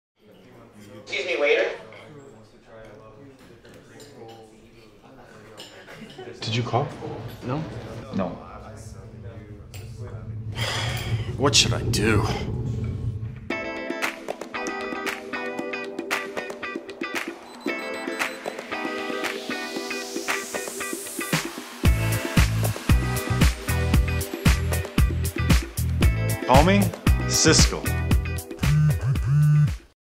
SYSCALL Σύστημα εξυπηρέτησης πελατών
Οι σερβιτόροι θα γνωρίζουν από ποιο τραπέζι ήρθε η κλήση, από τον αριθμό που εμφανίζεται στον δέκτη. Περισσότερα στο